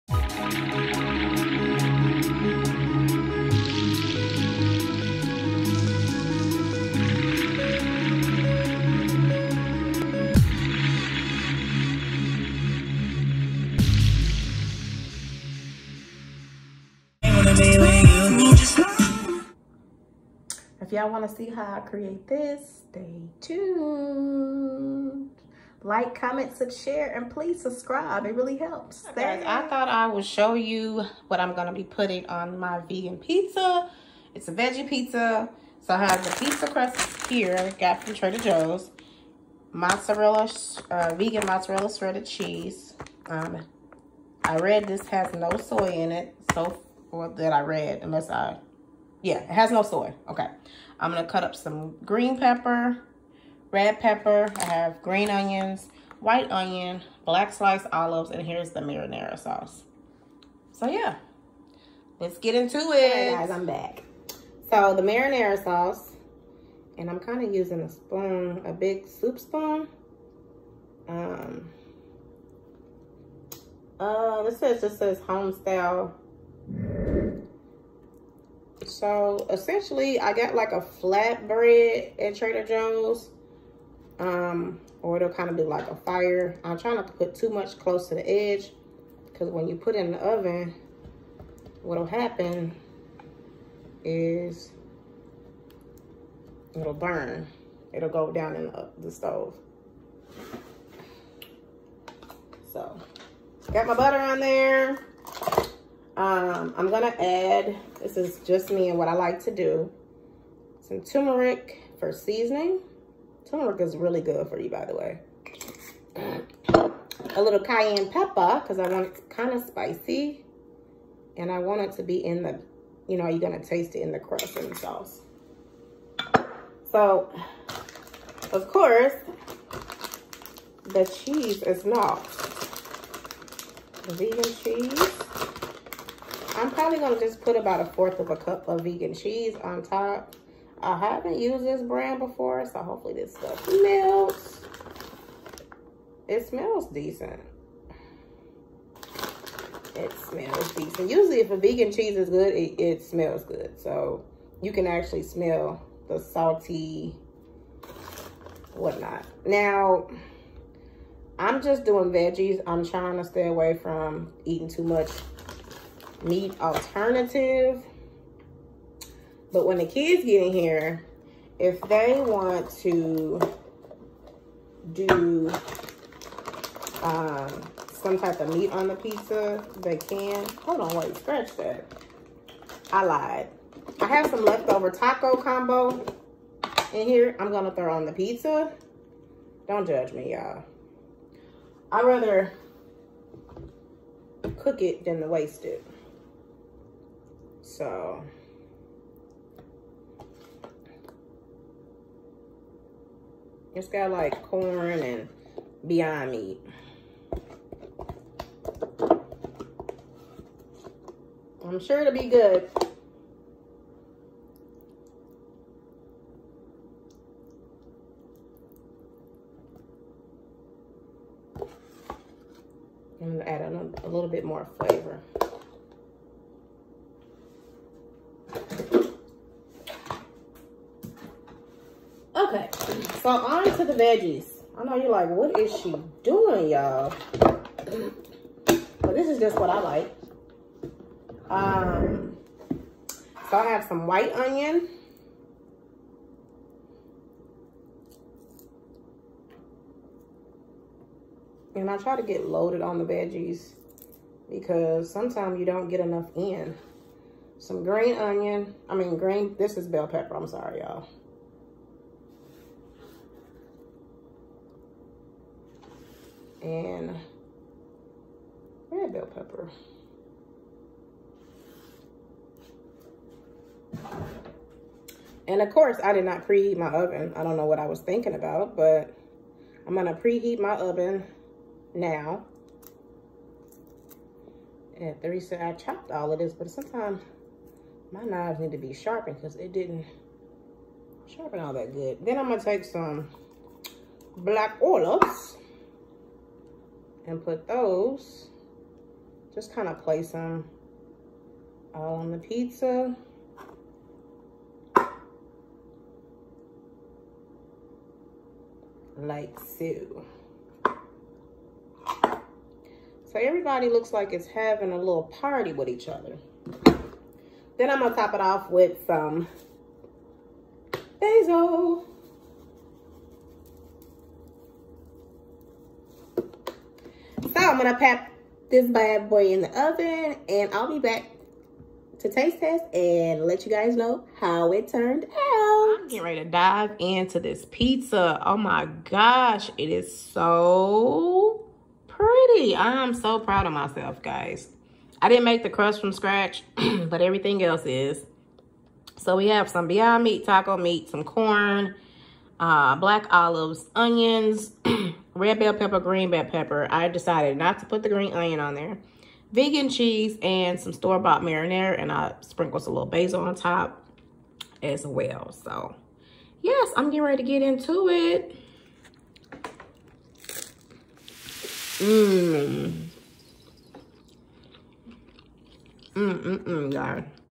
Vegan Flatbread Pizza | Prep Time 10 Minutes | Trader Joe’s Ingredients
Healthy Eating with clean delicious taste!

Many of you are wondering how to make a Vegan Pizza without breaking the bank. Here is a recipe that can be used for anyone Vegan, on a Diet, Lactose Free, and delicious taste. You must try this or recommend to your friends looking for alternatives . I hope you enjoy these type of videos. Peace !

Firewood FlatBread Pizza Crust
Vegan Mozzarella Cheese
Marinara Sauce
Canned sliced black olives
Canned Sweet Corn
Chopped White & Green Onions
 Beyond Meat
Red & Green Bell Peppers

Preheat oven 375' ( Cook Time 15-20)